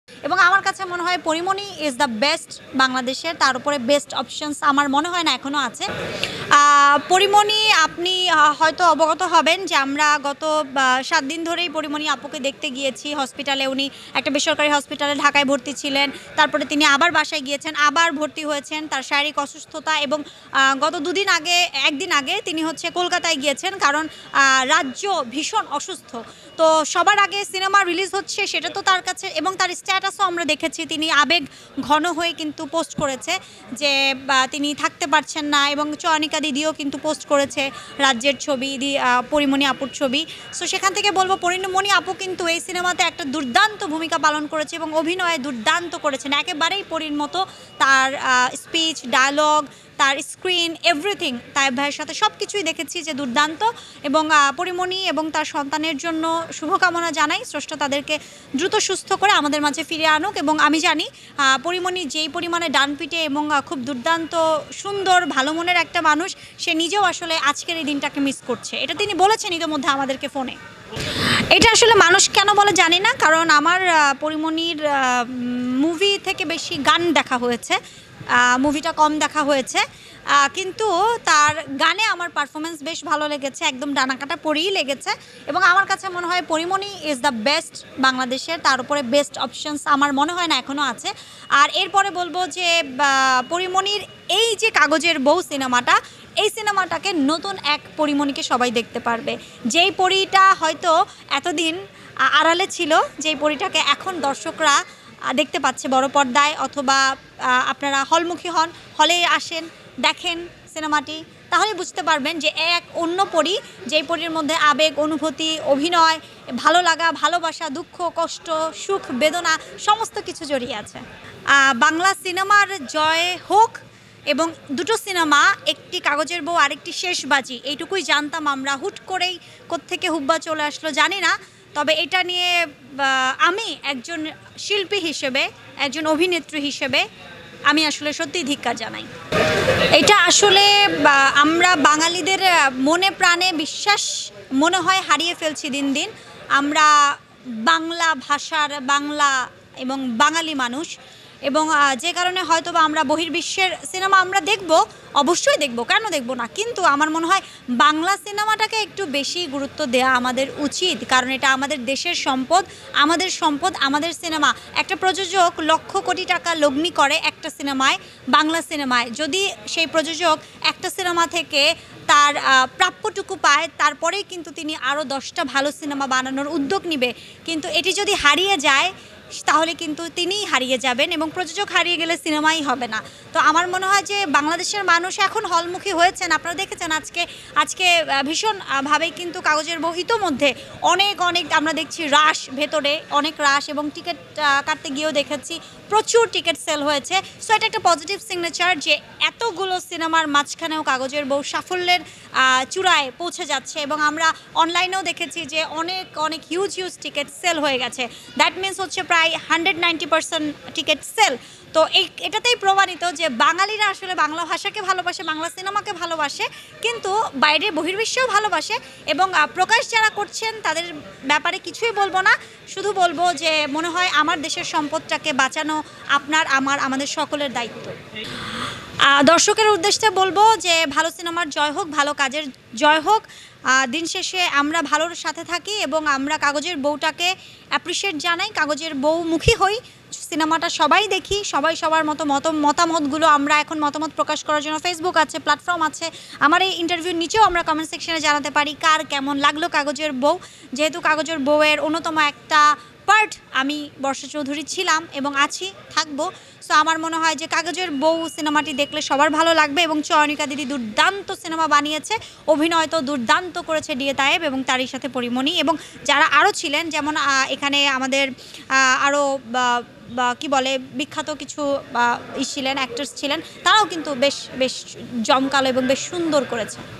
পরিমনি বাংলাদেশের সবচেয়ে ভালো নায়িকা । ডি এ তায়েব আর পরিমনিকে নিয়ে মুখ খুললেন বর্ষা ।
৫৯৫ টাকায় যেভাবে গরুর মাংস বিক্রয় করে খলিল সাহেব।আসল রহস্য উন্মোচন।